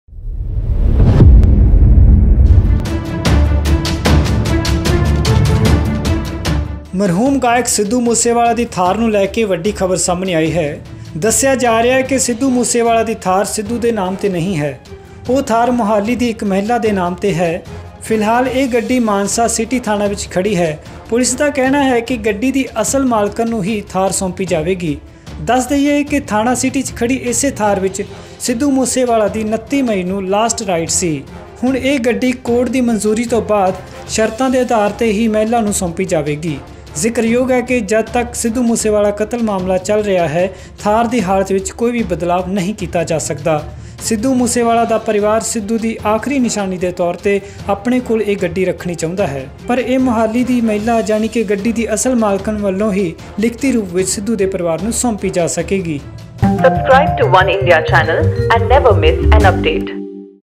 SidhuMoosewala ਦੀ ਲਾਸਟ ਰਾਈਡ ਵਾਲੀ Thar ਨੂੰ ਲੈਕੇ ਨਵਾਂ ਖੁਲਾਸਾ | OneIndia Punjabi
ਮਰਹੂਮ ਗਾਇਕ ਸਿੱਧੂ ਮੂਸੇਵਾਲਾ ਦੀ ਥਾਰ ਨੂੰ ਲੈ ਦੱਸਿਆ ਜਾ ਰਿਹਾ ਹੈ ਕਿ ਸਿੱਧੂ ਮੂਸੇਵਾਲਾ ਦੀ ਥਾਰ ਸਿੱਧੂ ਦੇ ਨਾਮ ਨਹੀਂ ਹੈ। ਉਹ ਥਾਰ ਮੁਹਾਲੀ ਦੀ ਇੱਕ ਮਹਿਲਾ ਦੇ ਨਾਮ 'ਤੇ ਹੈ। ਫਿਲਹਾਲ ਇਹ ਥਾਰ ਮਾਨਸਾ ਸਿਟੀ ਥਾਣਾ ਵਿੱਚ ਖੜੀ ਹੈ। ਪੁਲਿਸ ਦਾ ਕਹਿਣਾ ਹੈ ਕਿ ਗੱਡੀ ਦੀ ਅਸਲ ਮਾਲਕਣ ਨੂੰ ਹੀ ਥਾਰ ਸੋਂਪੀ ਜਾਵੇਗੀ। ਦਸ ਦਈਏ ਕਿ ਥਾਣਾ ਸਿਟੀ 'ਚ ਖੜੀ ਇਸੇ ਥਾਰ ਵਿਚ ਸਿੱਧੂ ਮੂਸੇ ਵਾਲਾ ਦੀ 29 ਮਈ ਨੂੰ ਲਾਸਟ ਰਾਈਡ ਸੀ। ਹੁਣ ਇਹ ਗੱਡੀ ਕੋਰਟ ਦੀ ਮਨਜ਼ੂਰੀ ਤੋਂ ਬਾਅਦ ਸ਼ਰਤਾਂ ਦੇ ਅਧਾਰ 'ਤੇ ਮਹਿਲਾ ਨੂੰ ਸੌਂਪੀ ਜਾਂਵੇਗੀ। ਜਿਕਰਯੋਗ ਹੈ ਕਿ ਜਦ ਤੱਕ ਸਿੱਧੂ ਕਤਲ ਮਾਮਲਾ ਚਲ ਰਿਹਾ ਹੈ ਥਾਰ ਦੀ ਹਾਲਤ ਵਿੱਚ ਕੋਈ ਬਦਲਾਵ ਨਹੀਂ ਕੀਤਾ ਜਾ ਸਕਦਾ। ਸਿੱਧੂ ਮੂਸੇਵਾਲਾ ਦਾ ਪਰਿਵਾਰ ਸਿੱਧੂ ਦੀ ਆਖਰੀ ਨਿਸ਼ਾਨੀ ਦੇ ਤੌਰ 'ਤੇ ਆਪਣੇ ਕੋਲ ਇਹ ਗੱਡੀ ਰੱਖਣਾ ਚਾਹੁੰਦਾ ਹੈ ਪਰ ਇਹ ਮੋਹਾਲੀ ਦੀ ਮਹਿਲਾ ਜਾਣੀ ਗੱਡੀ ਦੀ ਅਸਲ ਮਾਲਕਣ ਵੱਲੋਂ ਹੀ ਲਿਖਤੀ ਰੂਪ ਵਿੱਚ ਸਿੱਧੂ ਦੇ ਪਰਿਵਾਰ ਨੂੰ ਸੋਂਪੀ ਜਾ ਸਕੇਗੀ।